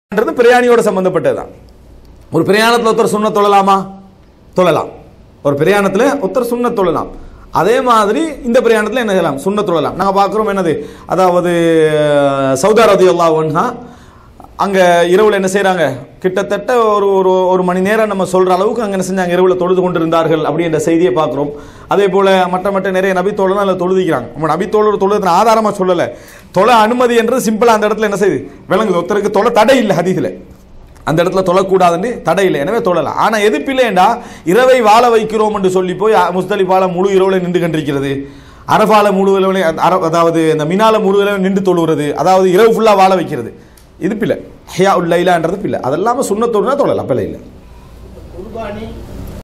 ஹஜ் கடமையின் போது சுன்னத் தொழ முடியுமா?┇Sheikh Mujahid Bin Razeen
ஹஜ் பற்றிய கேள்வி மற்றும் பதில் நிகழ்ச்சி ஜுபைல்.
4-8-2017 வெள்ளிக் கிழமை.
ஒளிப்பதிவு: சகோதரர் ஹனீபா.